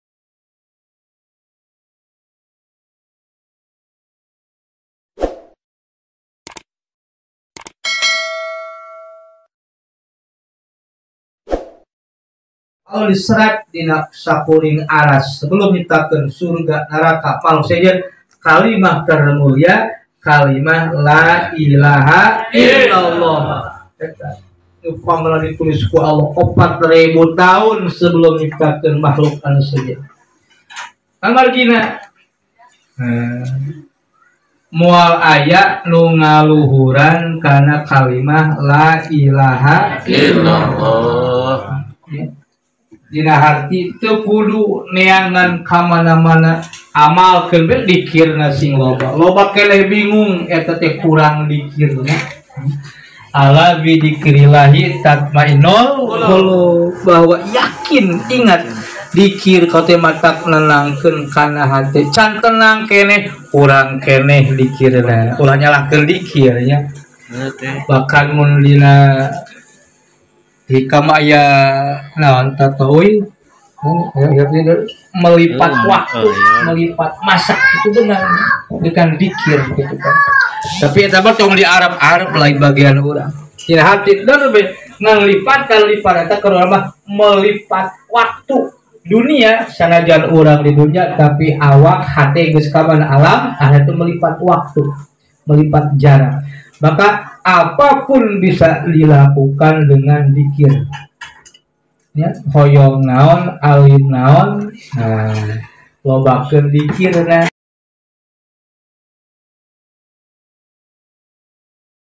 Doa Pelunas Hutang
Cara melunasi hutang dengan hati tenang dan enjoy tiada doa yang lebih ampuh yaitu dengan banyak dzikir..ucapan lailahhaillallah.